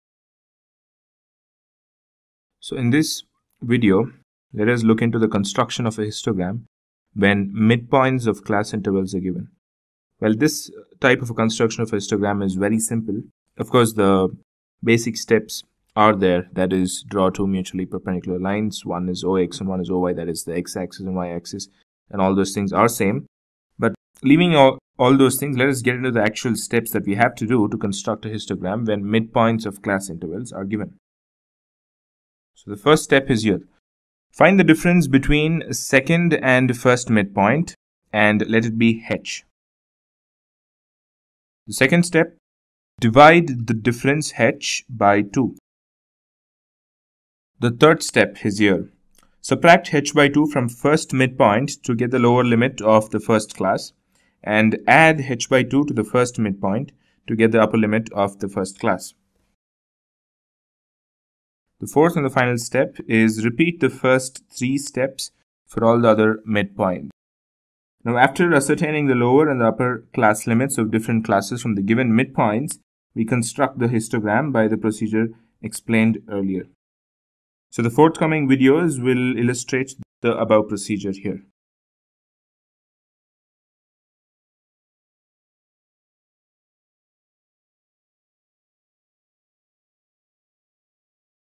How to construct histograms when mid points of class intervals are given ?
This video explains method of constructing a histogram when mid points of class intervals are given.